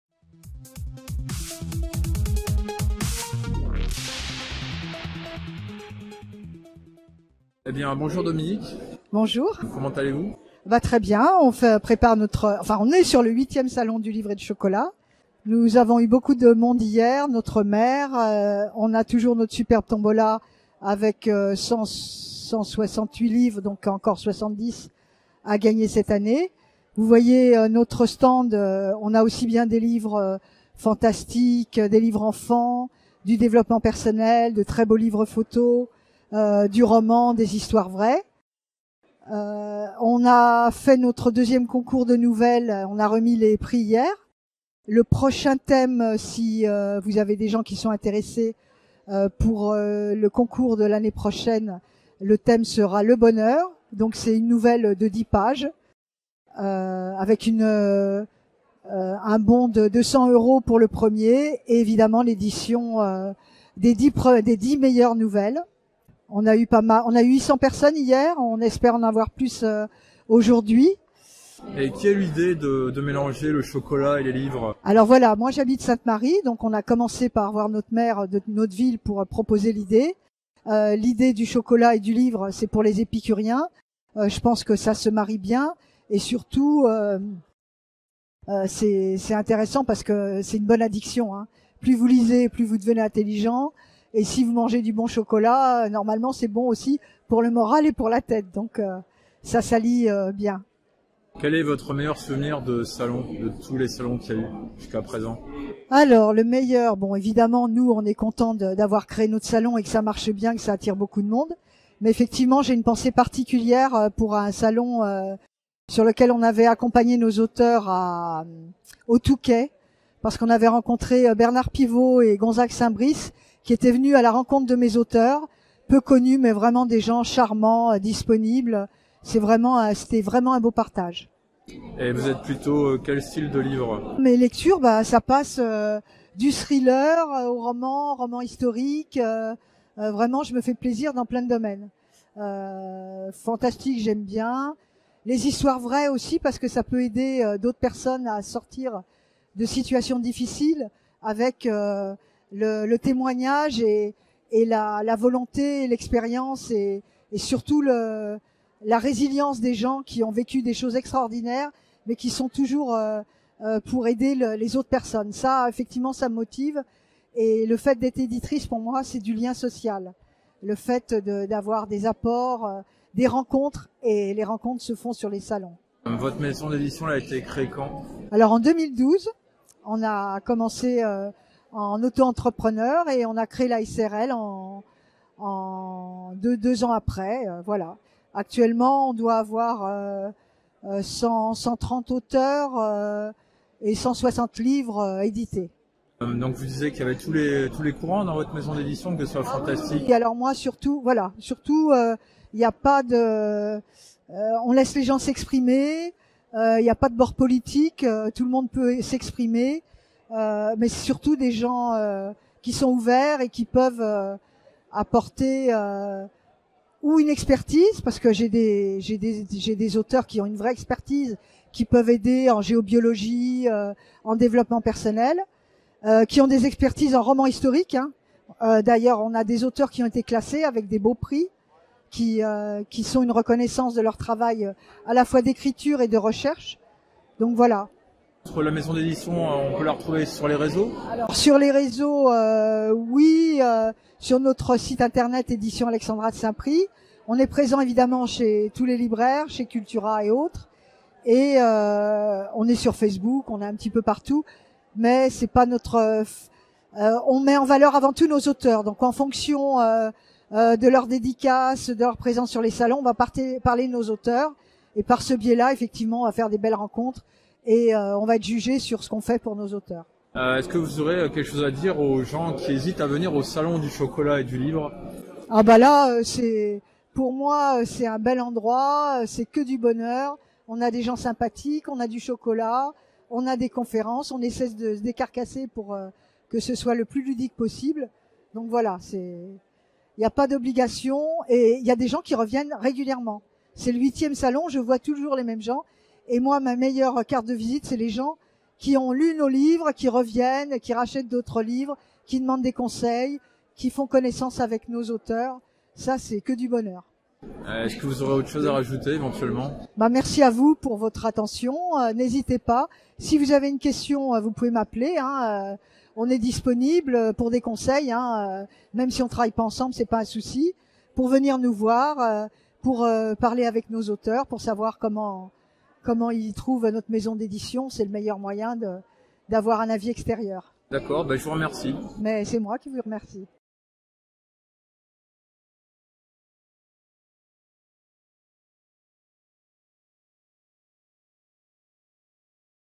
Les entretiens de Zone Imaginaire - Dominique DOYEN
[Avertissement]
Bonjour, malheureusement des problèmes techniques durant le tournage ont affectés le son pour cette vidéo. Il se peut que la vidéo soit réenregistrée prochainement.
Nous sommes désolés pour l'inconvénient.
Merci de votre compréhension.
La modératrice.

Les entretiens de Zone Imaginaire 113
Invitée : Dominique DOYEN (éditions Alexandra de Saint-Prix)

Animé par Gilles Thomas

(Une interview réalisée au cours de la 8ème édition du Salon du Livre et du Chocolat, le 29 octobre 2023 à Sainte Marie la Mer dans le département des Pyrénées-Orientales).

Retrouvez Zone Imaginaire sur